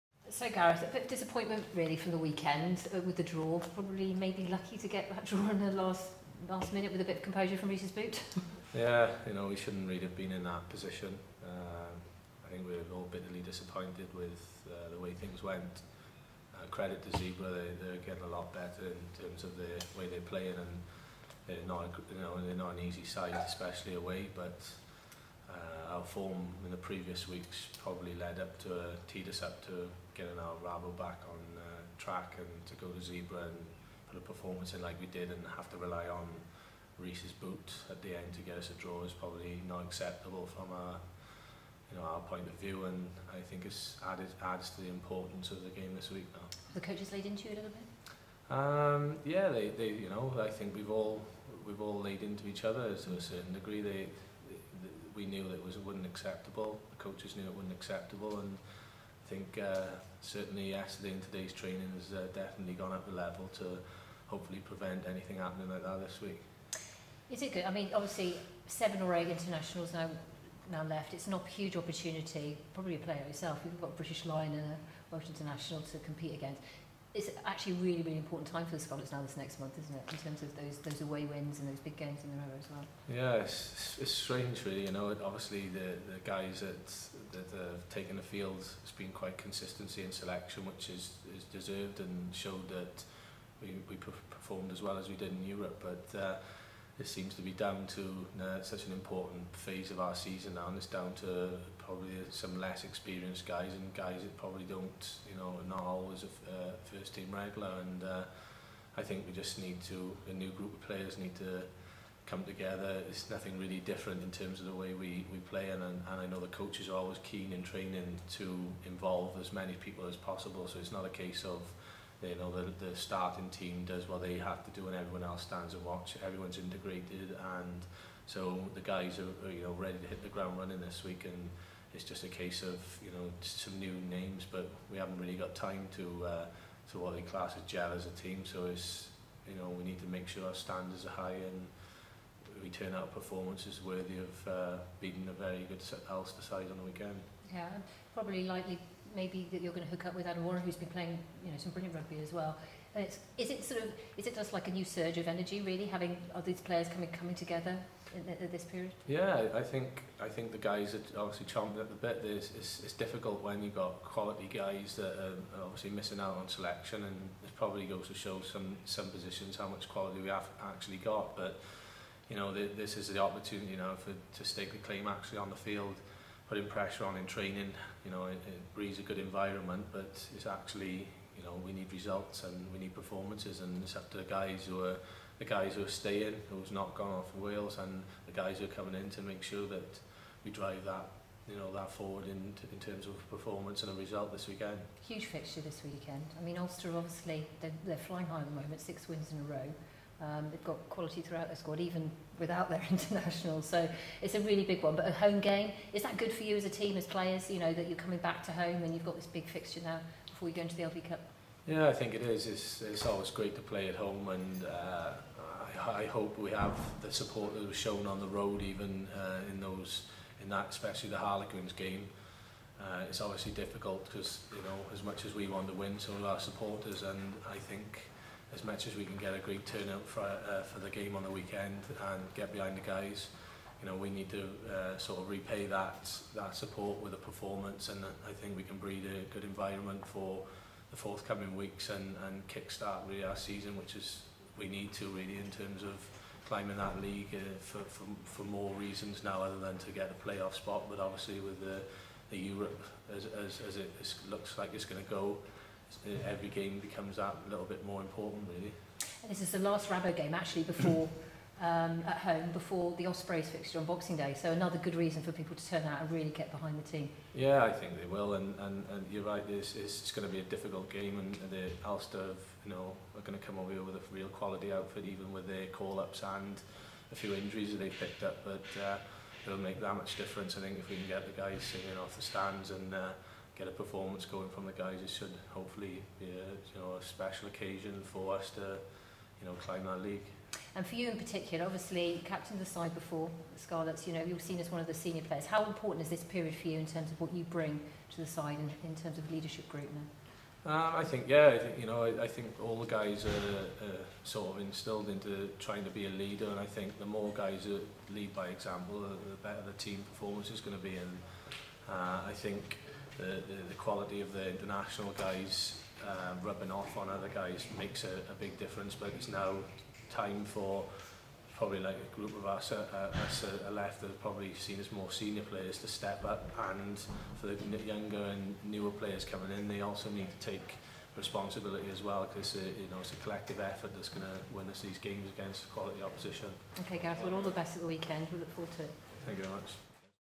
2013 10 29 - Gareth Maule
Scarlets centre Gareth Maule looks ahead to the RaboDirect PRO12 fixture against Ulster at Parc y Scarlets on Saturday 2nd November, kick-off 6.30pm